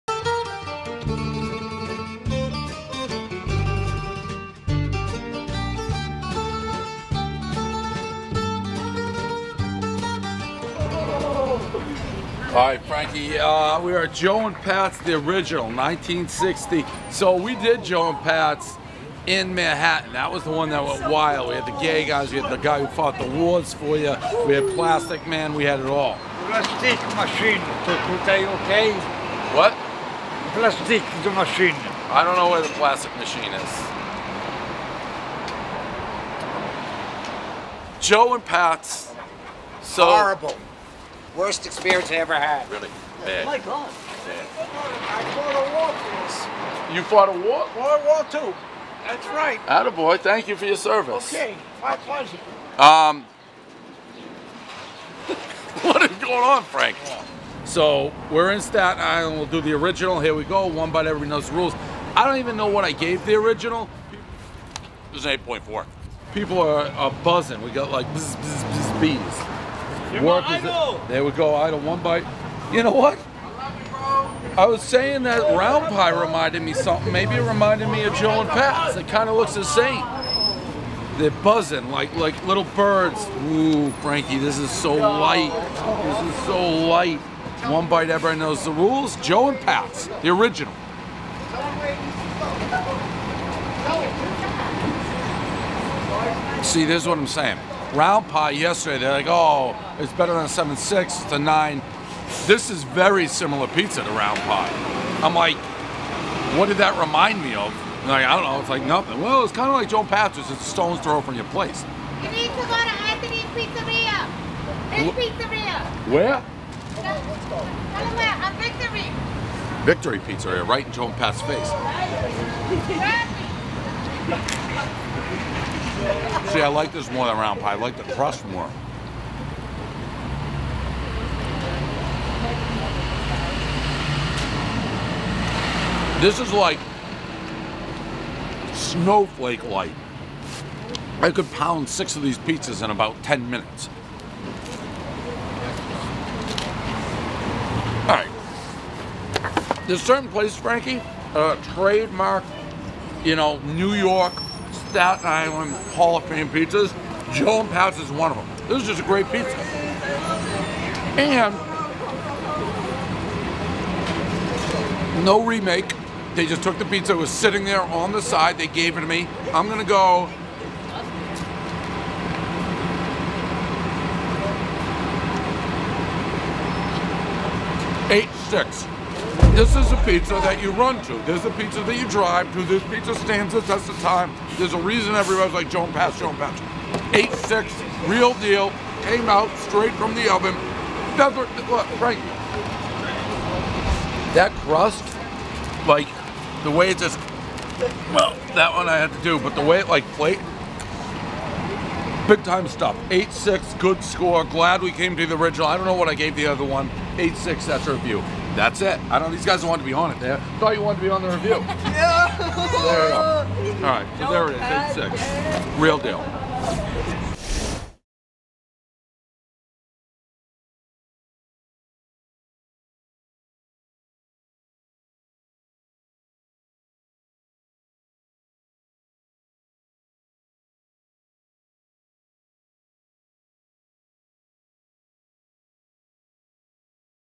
Barstool Pizza Review - The Original Joe & Pat's (Staten Island)
Dave visits the Original Joe & Pat's on Staten Island.